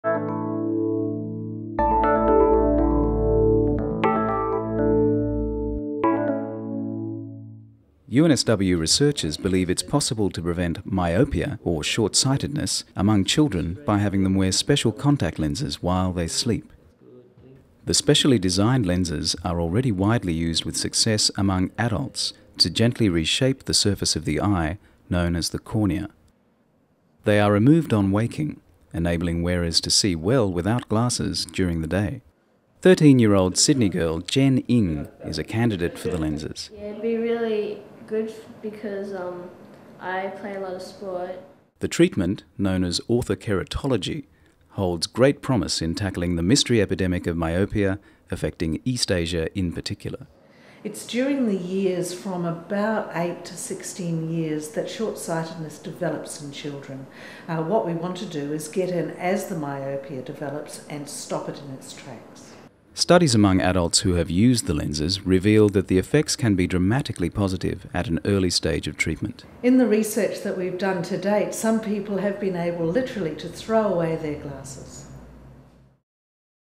Research focus on short-sighted kids
A new UNSW research project aims to discover whether special contact lenses worn only during sleep may slow or halt the progression of short-sightedness -- myopia -- in children.